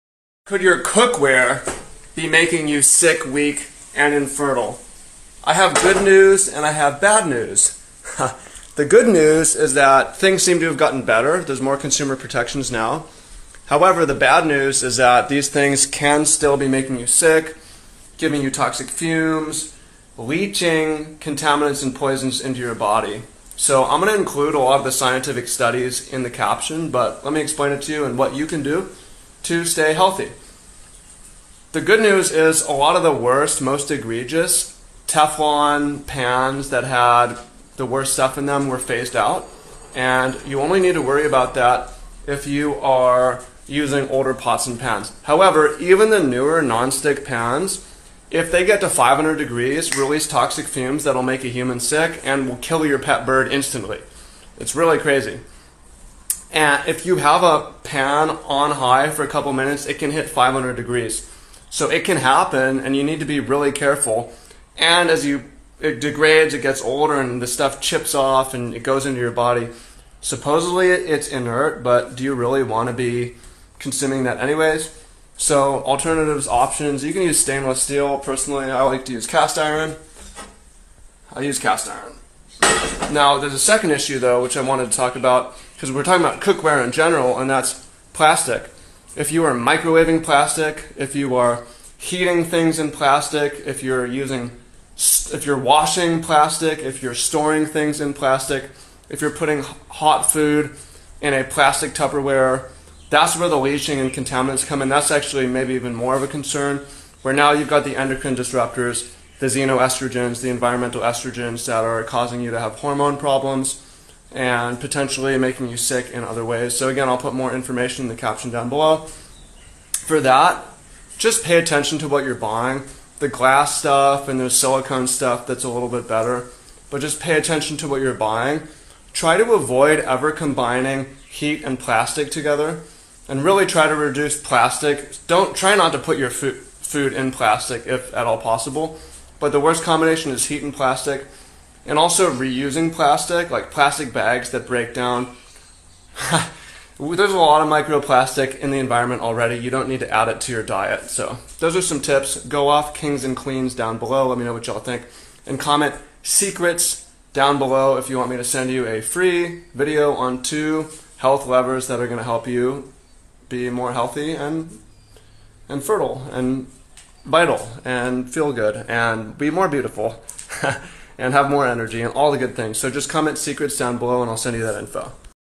Is your cookware making you sick, weak, or infertile?
Is your cookware making you sick, weak, or infertile? 😳 Maybe. Here's the scoop ⤵

Good news: Consumer protections have improved—many of the most harmful chemicals in Teflon and other non-stick pans were banned around 2013.
Bad news: New pans still pose risks. Overheat them above ~500 °F (260 °C) and you release PTFE fumes that can kill pet birds in minutes and give you "polymer-fume fever"—flu-like symptoms that are on the rise. There were 267 suspected cases in 2023, the most reported since 2000.

Plus, as the coating ages, chipping can occur—and while PTFE flakes are inert, most of us prefer not to ingest random chemicals.

Alternatives? Cast iron, stainless steel, ceramic—durable, safer, and better for your health.

Bigger issue alert: plastics. Plastic storage containers and bags are loaded with endocrine-disrupting chemicals—like BPA, BPS, and phthalates—that are linked to hormone disruption, poor fertility, and infertility .

The worst offenders: heating plastic—microwaving, dishwashing hot, putting hot food in them—or reusing damaged plastic. Heat + damaged plastic = more chemical leaching. Avoid plastic when you can. Glass, stainless, or food-grade silicone are better picks.

Comment your best secret swaps below! I'll send you a free video with health-and-fertility hacks most people miss. See you on the other side. ��